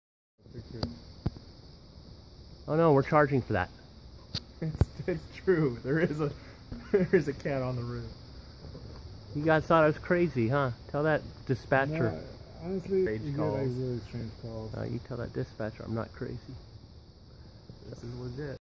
Bobcat Having Pigon Dinner On My Roof At 3am 911
Called 911 this morning as it sounded like someone was breaking in my house like a mad man.  Cops came rushing and when we went out side, it turned out to be a Bobcat chasing down a pigeon on my roof.  It was one of the biggest I have ever seen and he made an amazing jump over a tree into the neighbors yard from the roof when the officer pitched a rock at him.  The Bobcat was content just taking a nap.   Luck for our kitty that was in the backyard on the chair the Bobcat already had it's meal.